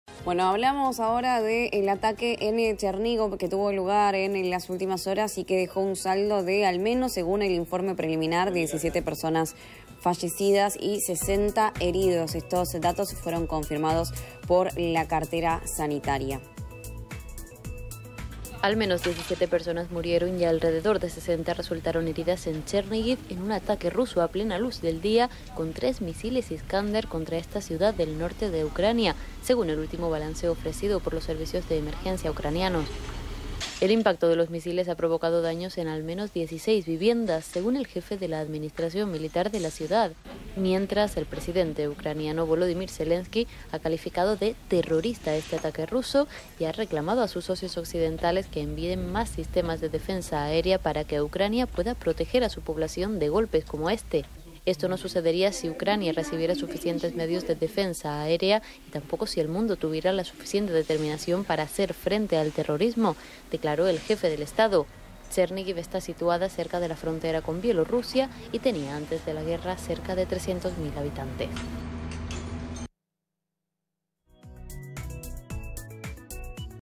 GUERRA RUSIA - UCRANIA | 17 muertos en el ataque del Kremlin con misiles contra Chernígov
👉Al menos 17 personas murieron y alrededor de 60 resultaron heridas en Chernígov en un ataque ruso a plena luz del día con tres misiles Iskander contra esta ciudad del norte de Ucrania, según el último balance ofrecido por los servicios de emergencia ucranianos.